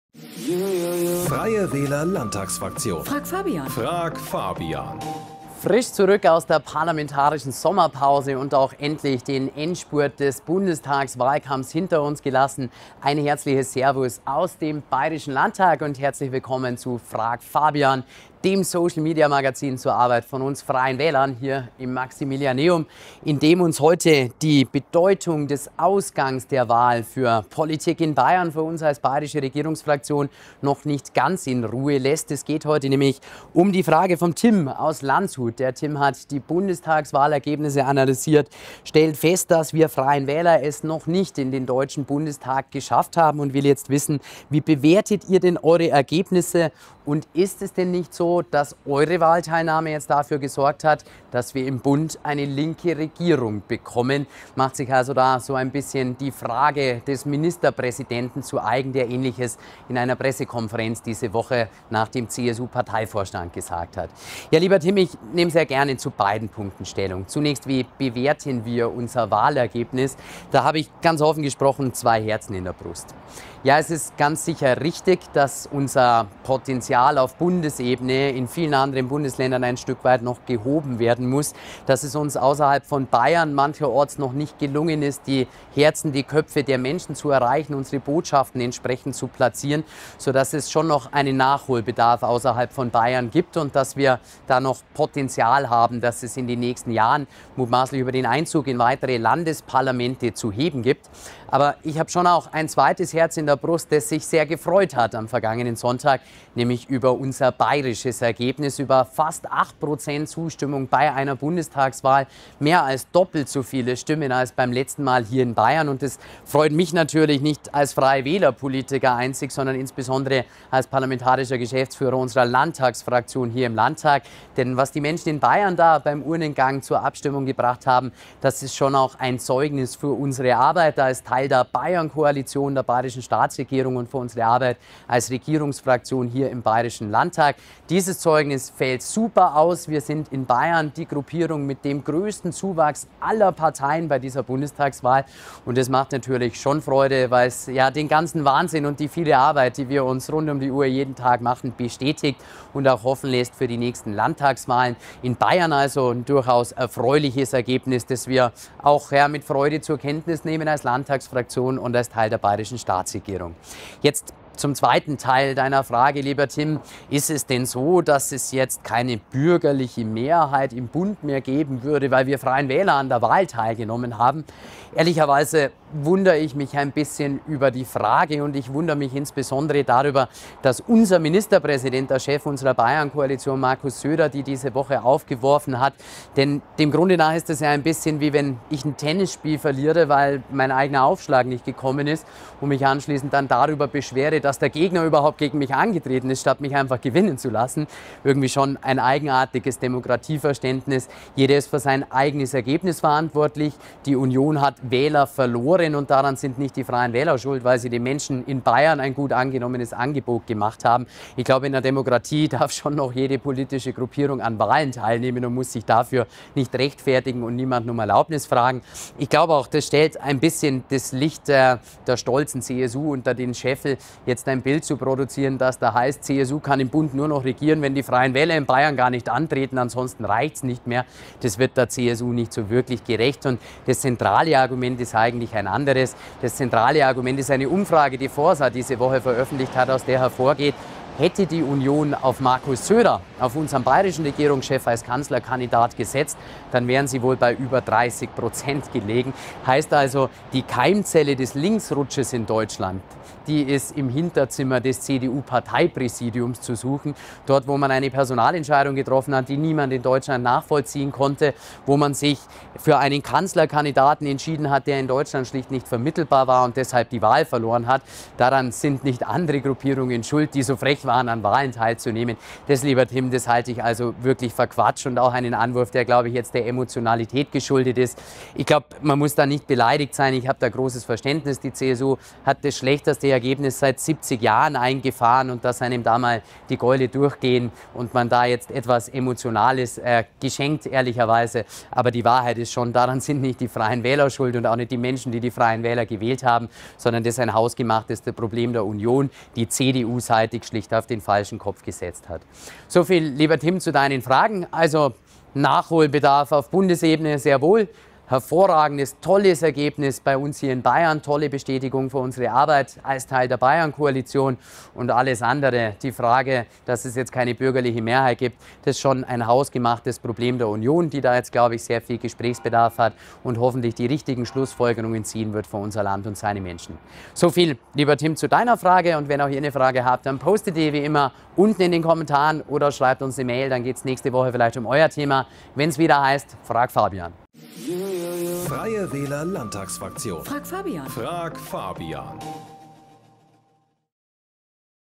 FRAGFABIAN zu den Ergebnissen der Bundestagswahl 2021
Die Bundestagswahl 2021 wirft ihre Schatten auch auf uns als Landtagsfraktion: Wie bewerten wir die Gesamtergebnisse? Wieso freuen uns die FREIEN WÄHLER-Stimmenanteile in Bayern? Und vor allem: Was hat das mit unserer Regierungsarbeit in der Bayernkoalition zu tun? - Frag' Fabian